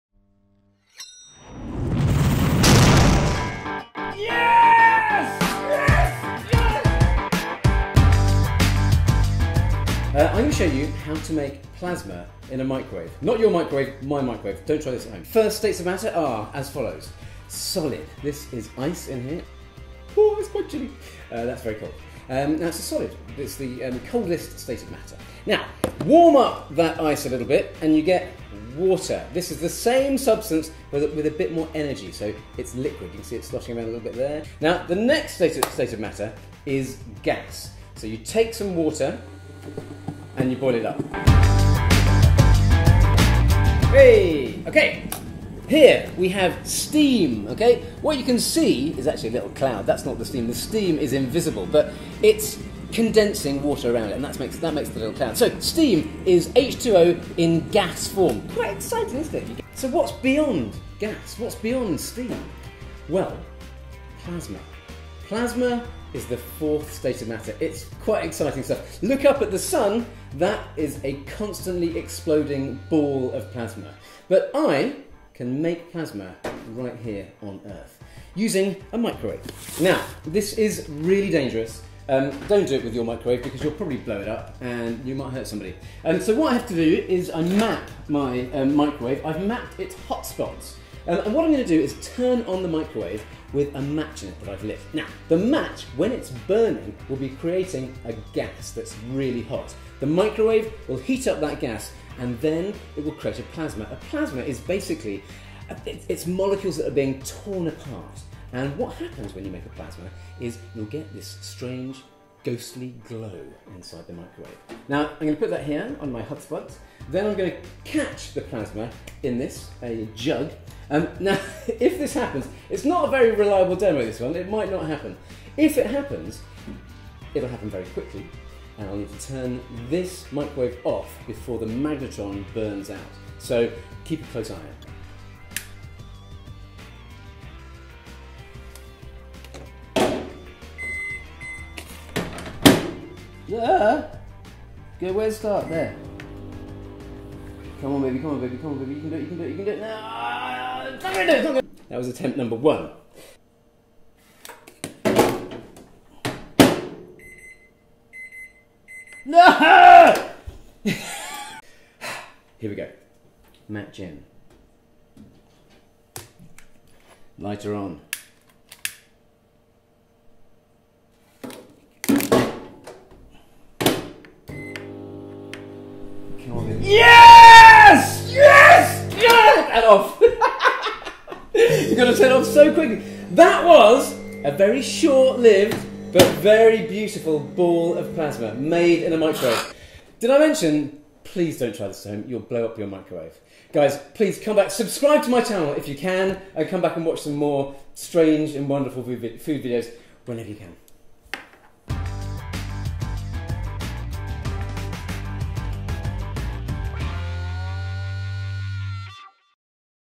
Wierdest recipe ever: How To Make Plasma in a Microwave
An amazing bit of physics - DON'T TRY THIS WITH YOUR MICROWAVE or you could damge it. Try it with mine!
This is about the states of matter, especially the fourth state - take a gas and superheat it until its molecules are torn apart by an excess of energy. That's plasma, my friends!